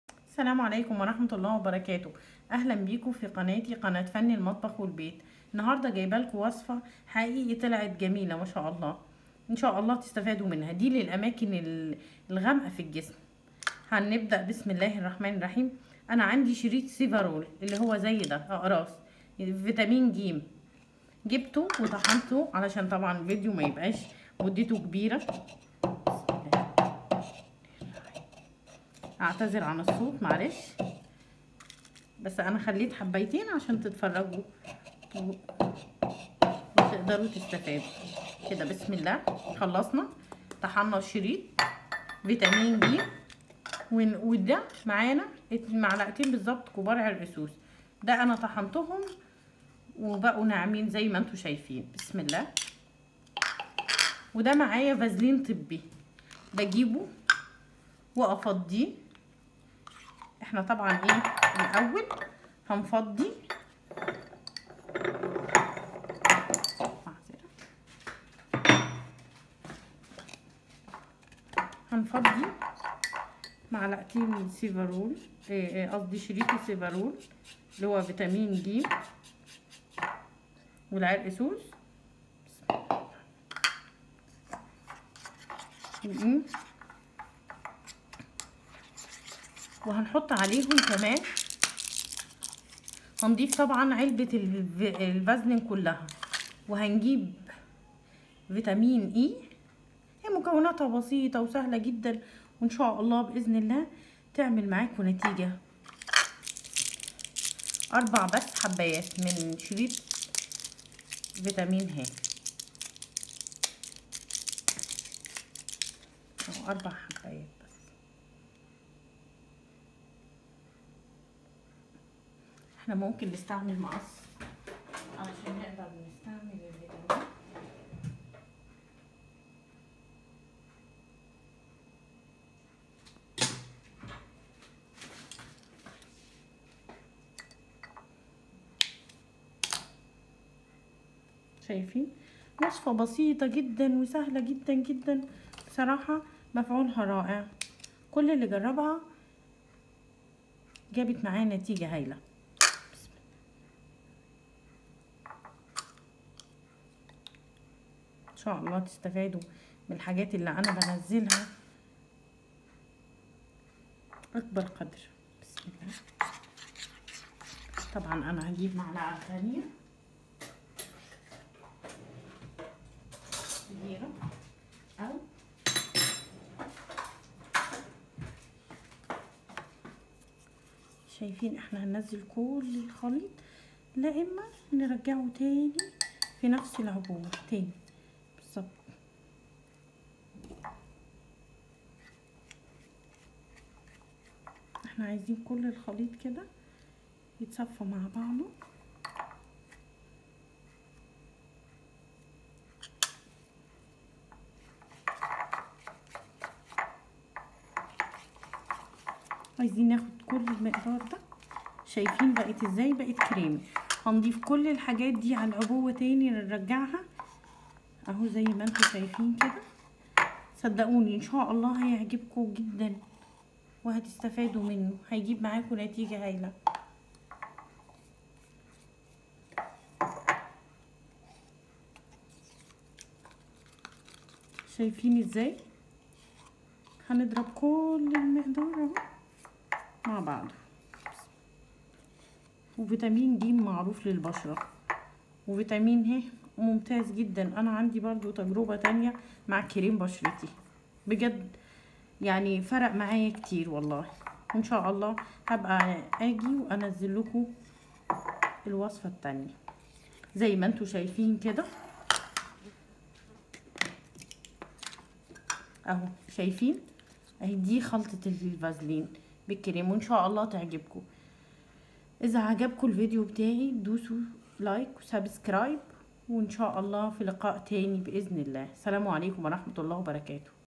الوصفه دي اخدتها من صيدلاني بيعمل تركيبات هتنور الاماكن الداكنه فن المطبخ والبيت مع وفاء السلايمه
الوصفه دي اخدتها من جارتي بس حلوه عن تجربه الوصفه عبارة عن شريط سيفارول ودة فيتامين ج و٢ معلقه عرق سوس مطحون  و٤ كبسولات فيتامين e وعلبه فازلين طبي
نطحن اقراص السيفارول ونضع عليهم العرق سوس  المطحون لازم ينخل  وبعدين نضيف عليهم الفازلين ونفتح كبسولات فيتامين e ونقلبه جدا ونضعها في علبه ونضع منها مرتين في اليوم  وان شا الله تستفدو منها اذا عجبكم الفيديو اضغطوا لايك وسبكريب والجرس ليصلكم كل جديد  تحياتي اليكم